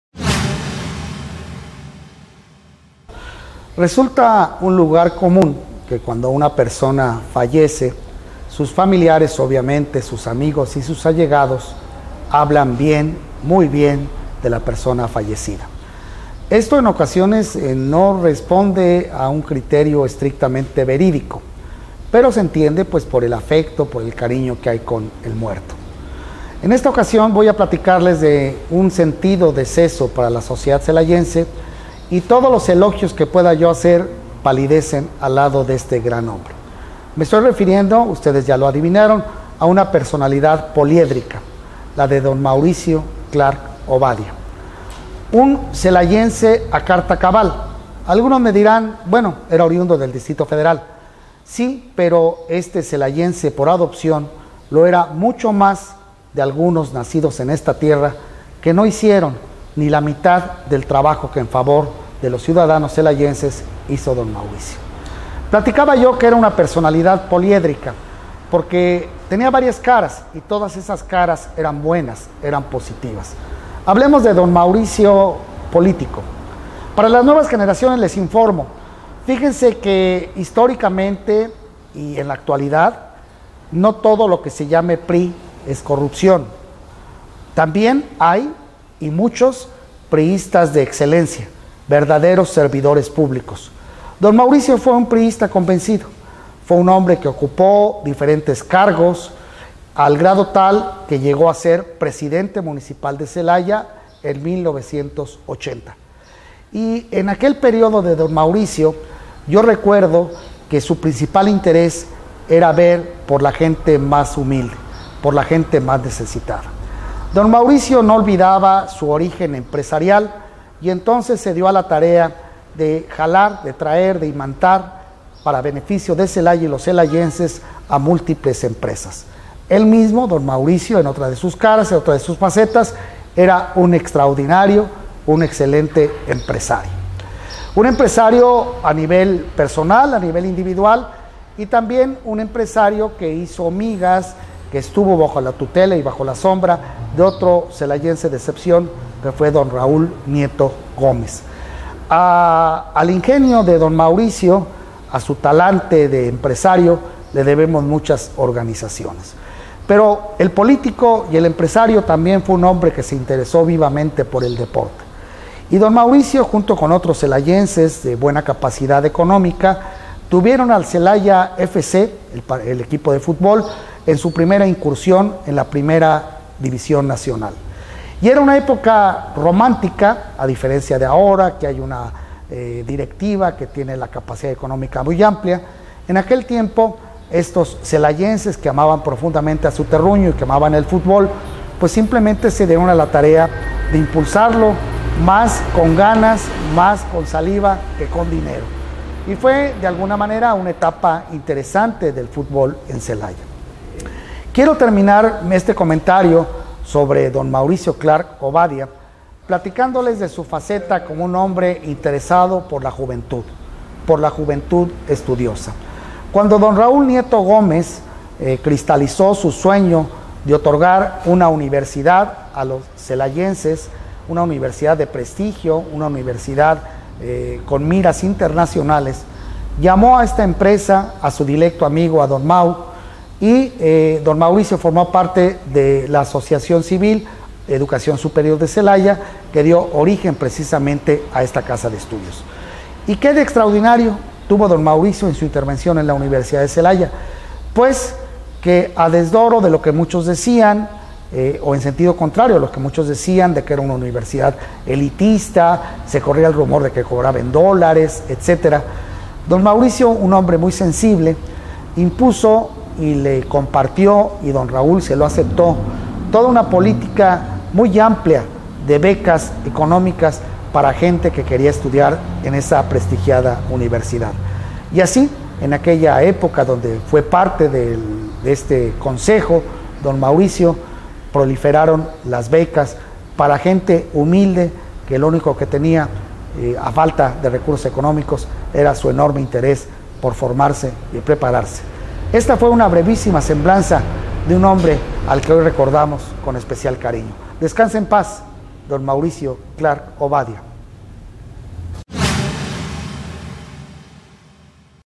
VALE SABER/ Lic. Fernando Amate
Cronista de Celaya dedica su espacio a la figura de un gran hombres al que Celaya sin duda recordará siempre, a don Mauricio Clark Ovadia, quien recién falleciera